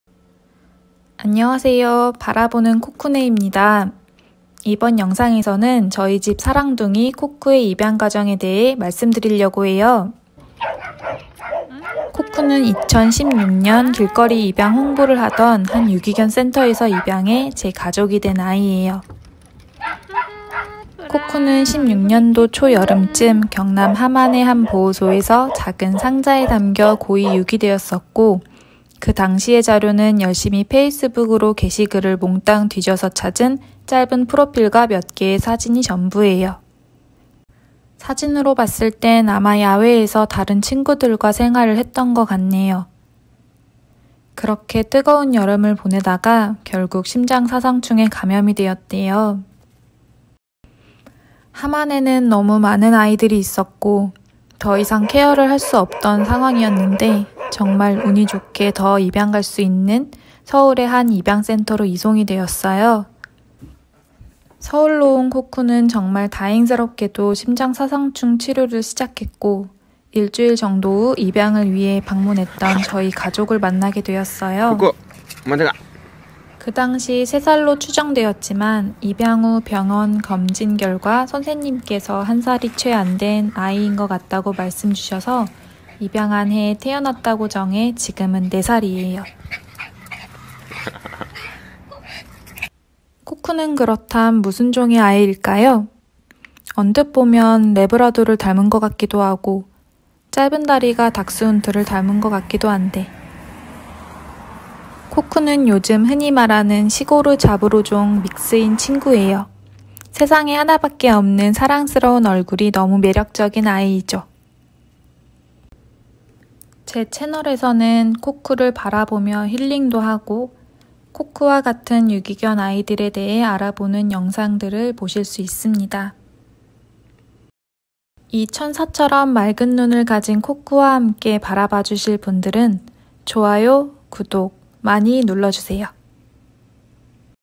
바라보는 코쿠ㅣ유기견 입양! 보호소에 살던 코쿠, 입양되어 가족이 되기까지 입양과정 전격공개
유기견입양 유기견보호소 시고르자브로종 유기견 출신 코쿠의 입양과정을 영상으로 만들어 보았습니다.

유기견 출신 코쿠의 입양 과정을 바라봐 주세요.
1. 고의 유기된 코쿠의 과거
2. 혹독한 보호소 생활 중 심장사상충 감염
3. 치료와 입양을 위한 서울로 상경
4. 믹스친구인 코쿠의 얼굴 공개